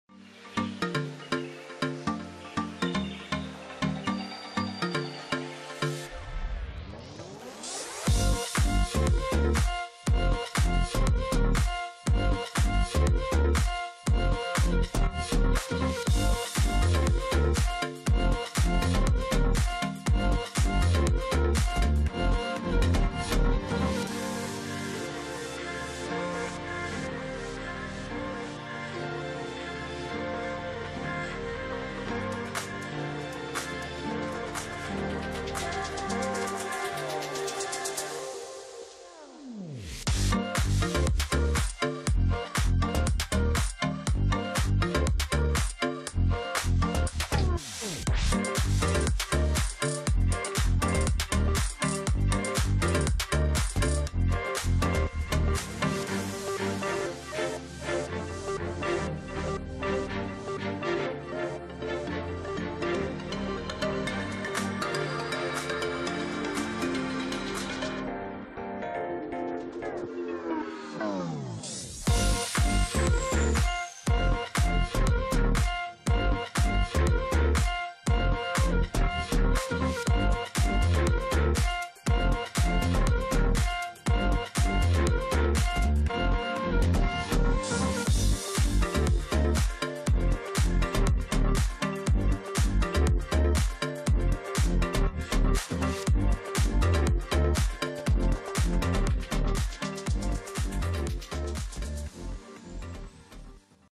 요즘 교양 필독서 87 _ 나가이 다카히사, 센시오 _ 교양인문 책​ 프리뷰
요즘 교양 필독서 87
철학부터 정치, 문화, 예술, 과학까지 지적 대화를 위한 교양 필독서 87권을 한 권에
나가이 다카히사 | 센시오
교양 | 고전 | 필독서 | 책추천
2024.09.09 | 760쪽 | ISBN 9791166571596